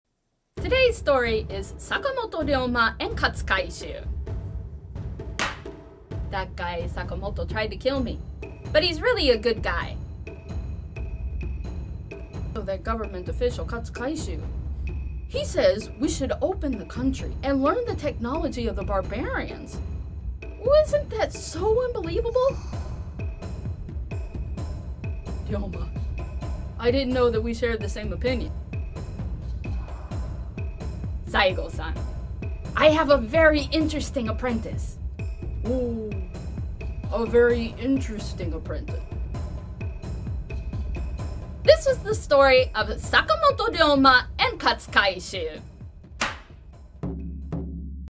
Introducing the characters of the Kodan: Sakamoto Ryoma and Katsu Kaishu
Kodan storytelling is a traditional Japanese performing art.  The stories we tell are based on history, and include historical figures and events.  Do you know who these samurai are?
Kodan: Sakamoto Ryoma and Katsu Kaishu will be performed at the virtual sekitei, Tenpachido Hall, on January 19th!